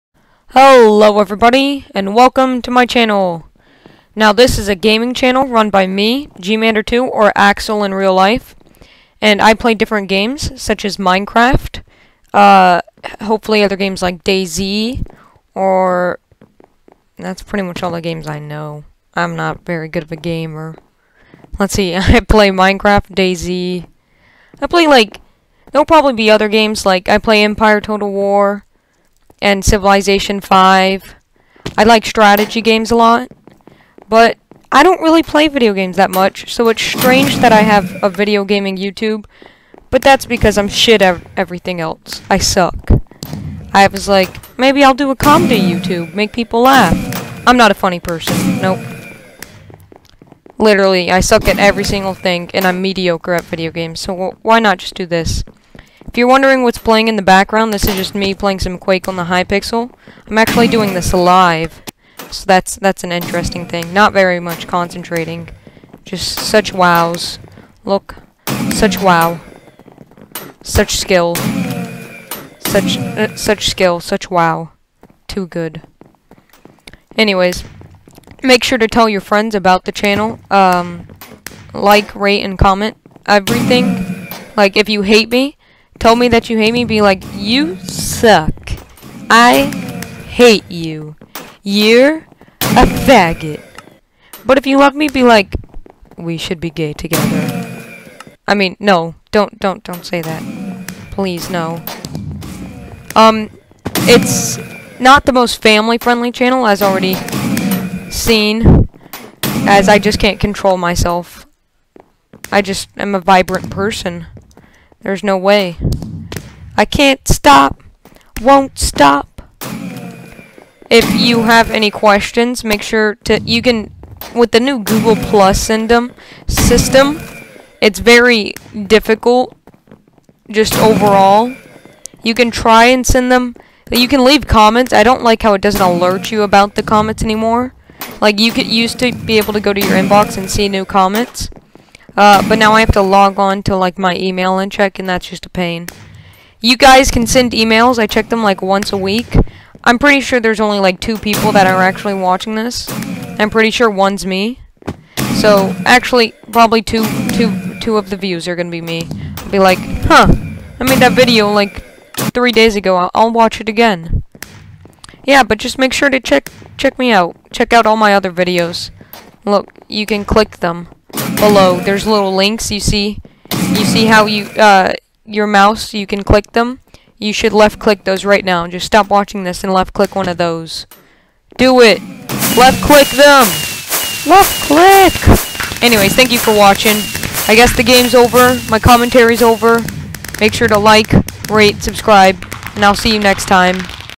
An Introduction!
Hello! Its been a good couple weeks so I decided to introduce my self a little. Comment everything you hate or love. FEEDBACK NOW!